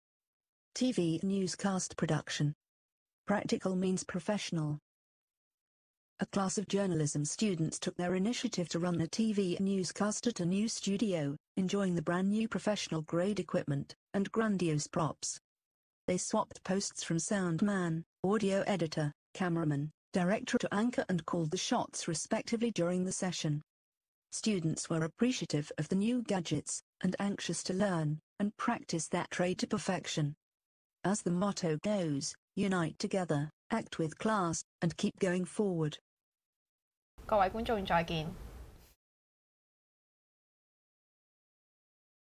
Practical Means Professional: Virtual Presenter 虛擬主播：練習乃成功之母
TV Newscast Production: Practical Means Professional
A class of journalism students took their initiative to run a TV newscast at our new studio, enjoying the brand new professional grade equipment and grandiose props.
They swapped posts from sound man, audio editor, cameraman, director to anchor and called the shots respectively during the session.
Students were appreciative of the new gadgets and anxious to learn and practice their trade to perfection.
As the motto goes: unite together, act with class and keep going forward.